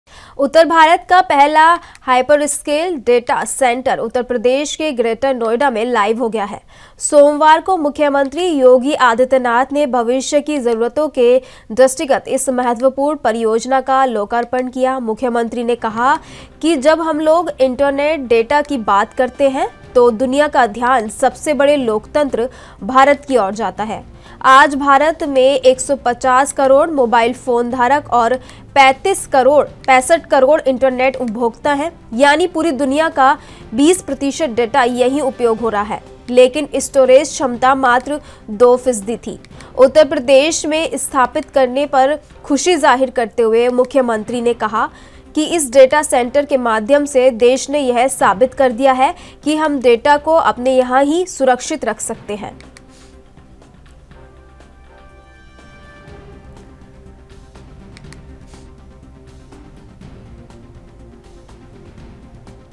ग्रेटर नोएडा को मिली डेटा सेंटर की सौगात | ANB NEWS
ग्रेटर नोएडा को मिली डेटा सेंटर की सौगात
CM ने यूपी के पहले डाटा सेंटर का किया लोकार्पण